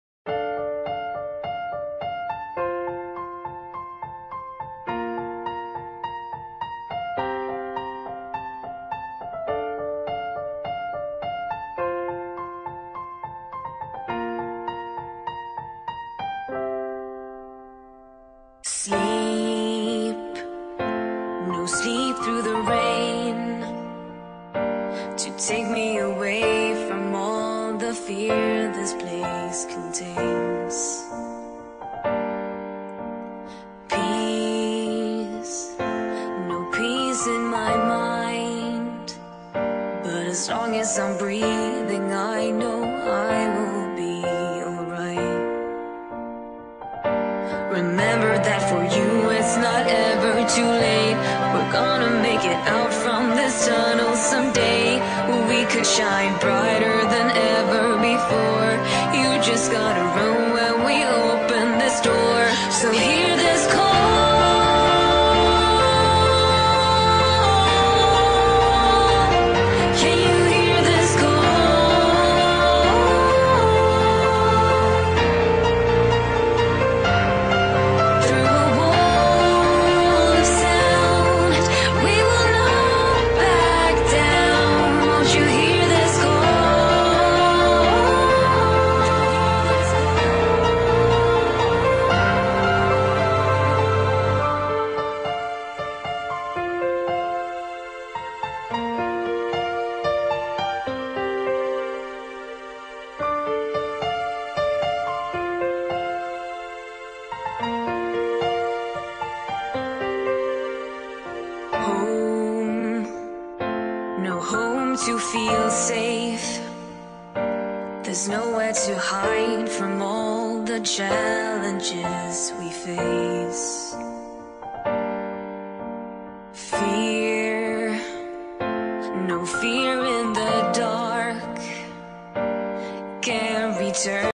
Hear This Call - till Clara Henry
hade inge att göra så jag gjorde denna te dig Clara O.o
för att visa din fina sida xD